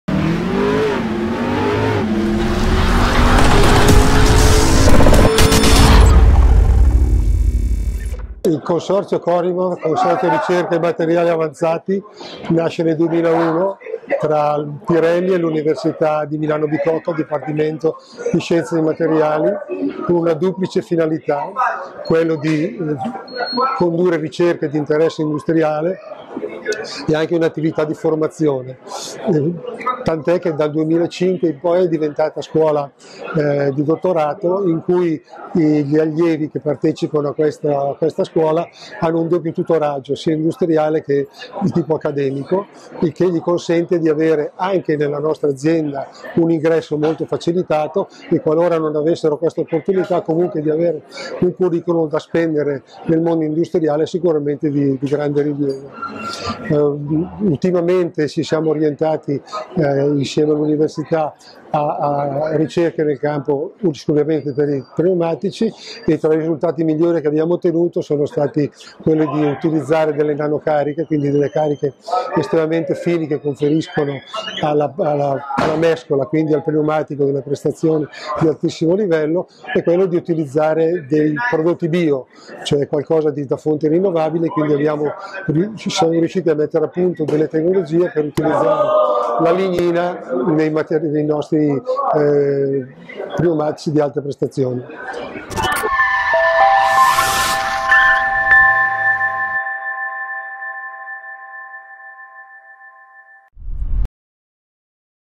Pirelli CORIMAV, l'intervista a Enrico Albizzati
Pirelli | Con il rinnovo del progetto CORIMAV tra Pirelli ed Università degli Studi di Milano Bicocca si protrarrà fino al 2022 un'intesa che porta il mondo del lavoro e quello dello studio ad incontrarsi. Finora i risultati sono stati eccellenti come ci spiega Enrico Albizzati,  Direttore CORIMAV e Scientific Advisor di Pirelli.

Servizio, riprese e montaggio di Andrea Villa